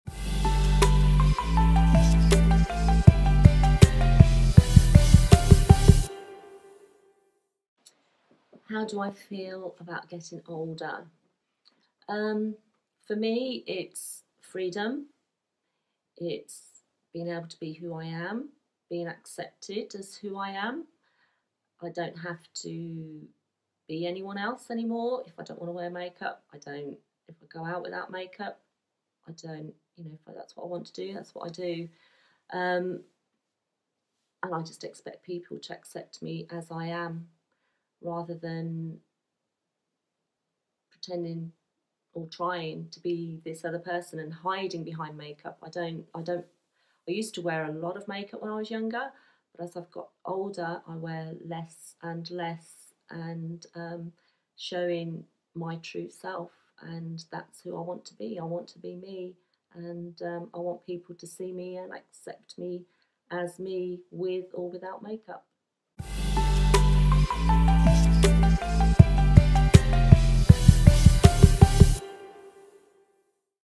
Lucia - How I feel about growing older
For Lucia, getting older means being accepted for who she is.
Part of the Face it, Own it! Book and Exhibition - Portraits and interviews with women 40+ without their make up, normalising seeing mature women as they are.